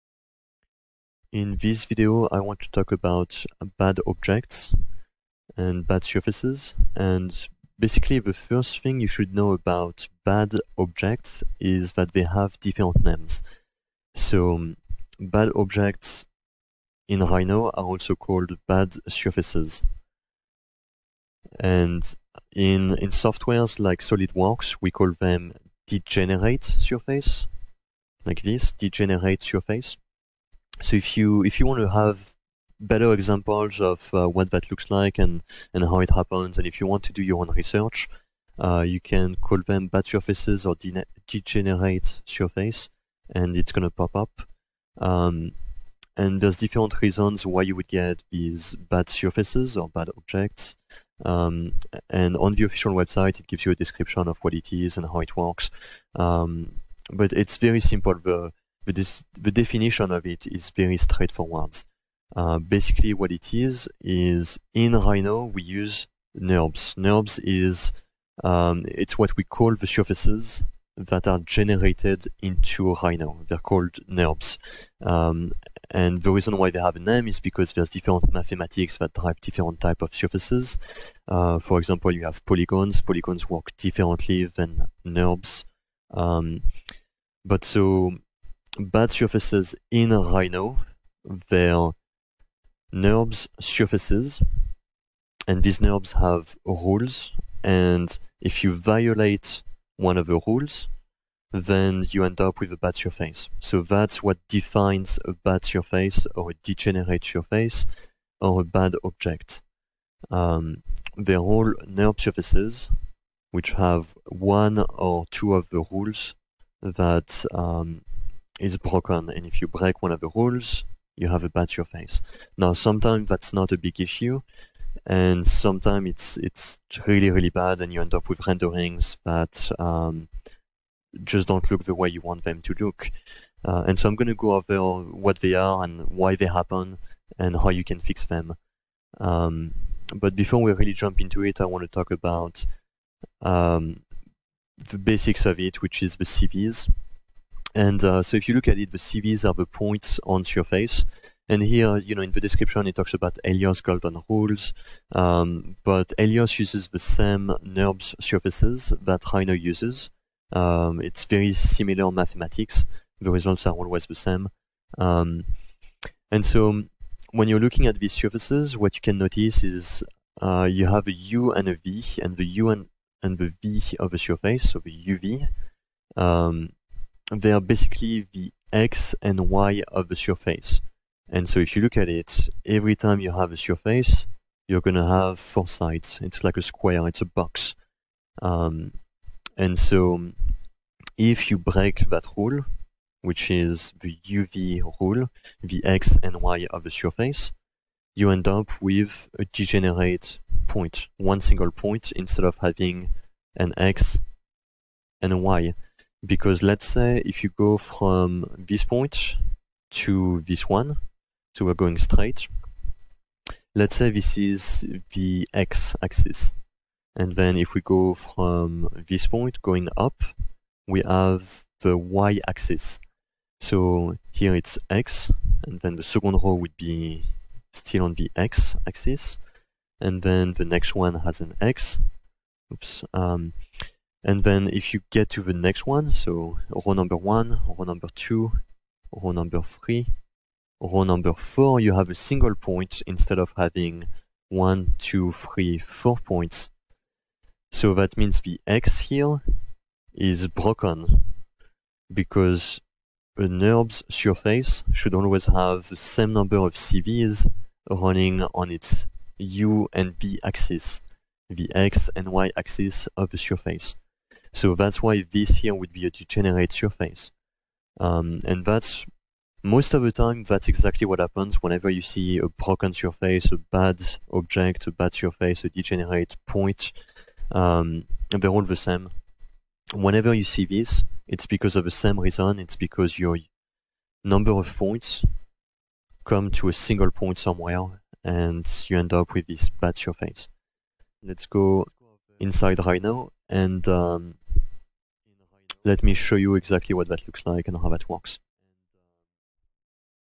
Rhino "Bad Objects" Part1
Overview of bad objects in Rhino. AKA bad surfaces. AKA degenerate surfaces.